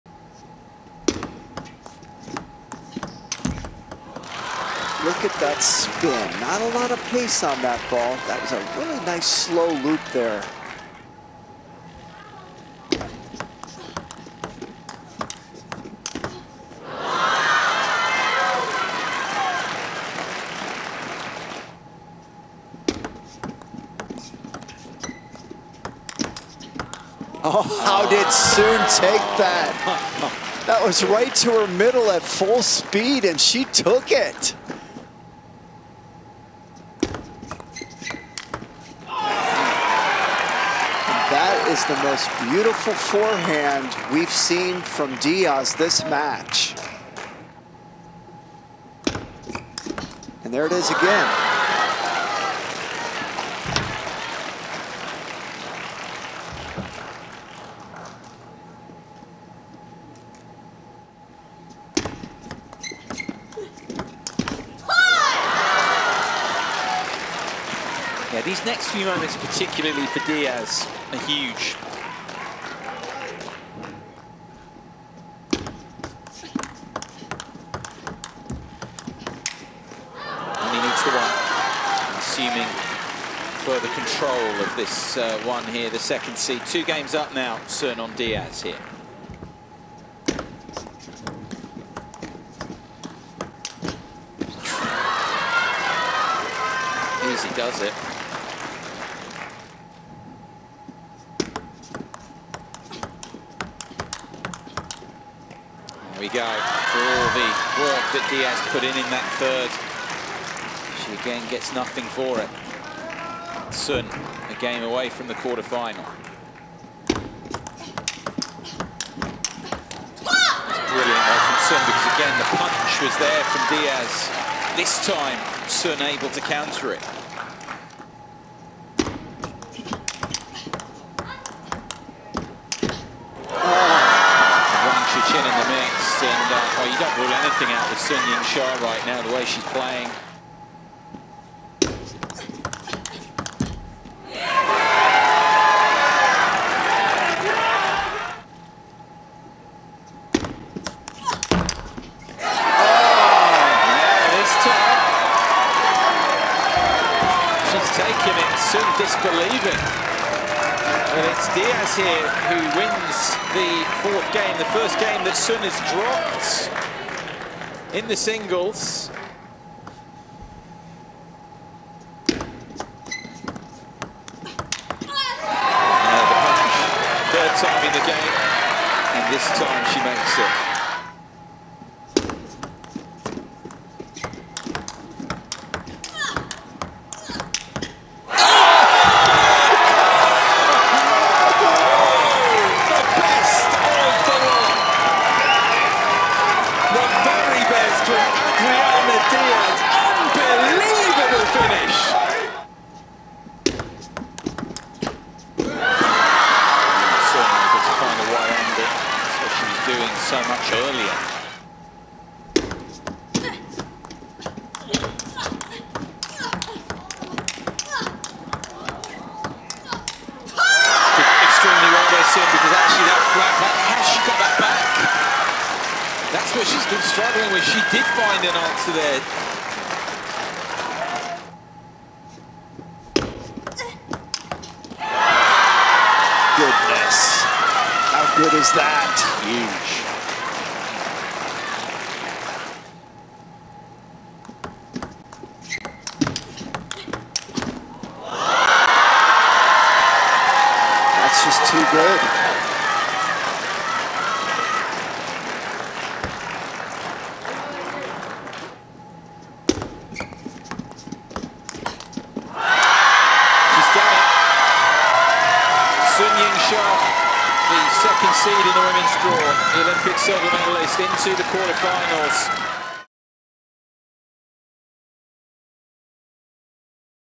Adriana Diaz vs Sun Yingsha | 2021 World Table Tennis Championships Finals | WS | R16
Sun Yingsha and Adriana Diaz put on quite the match at ITTFWorlds2021 with moves like these 🔥